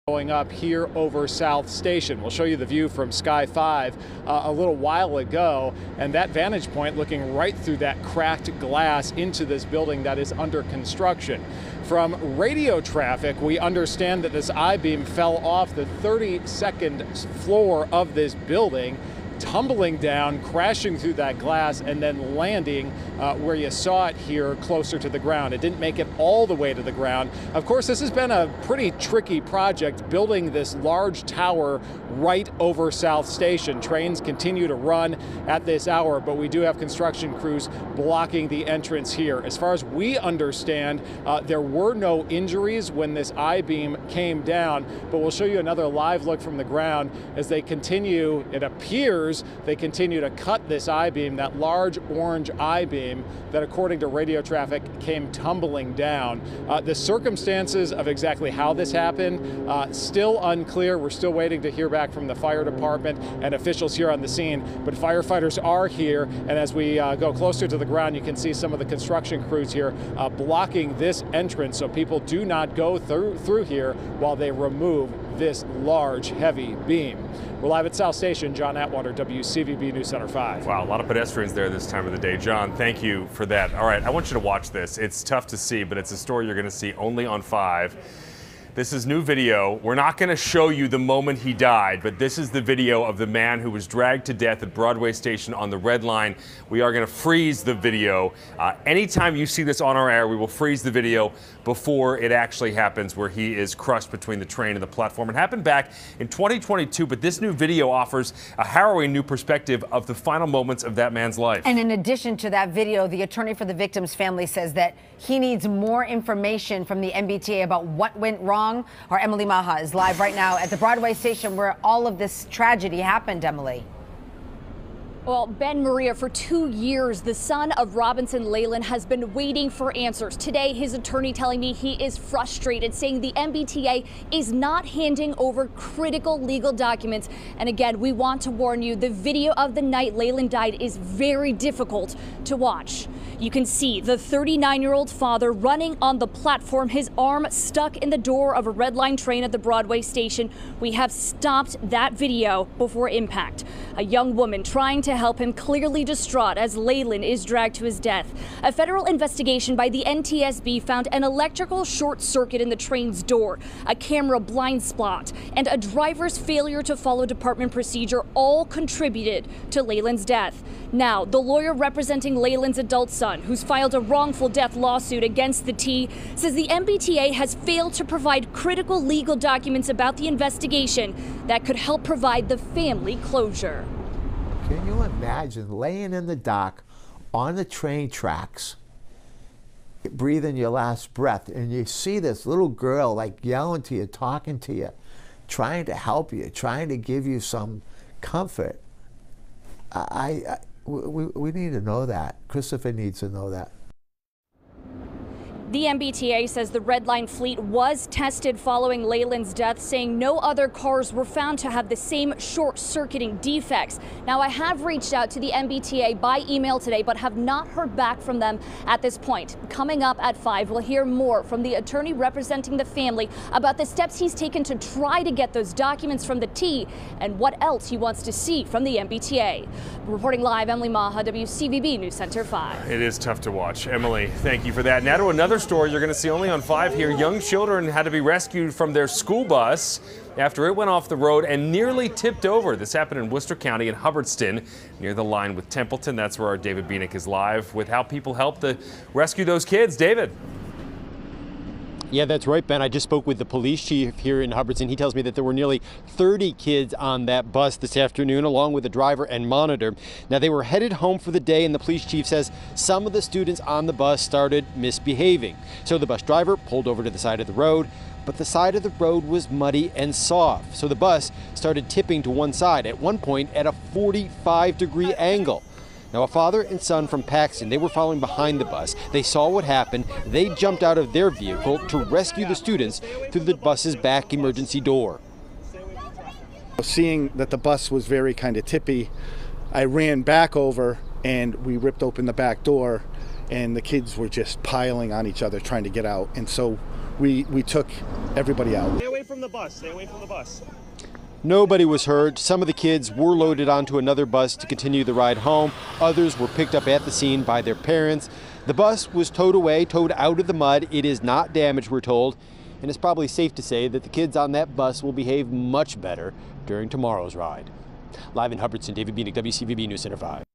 Students rescued after school bus sinks, tips in mud
Several students had to be evacuated off a school bus in central Massachusetts on Wednesday after the bus got stuck in the mud when it pulled over on the side of the roadway.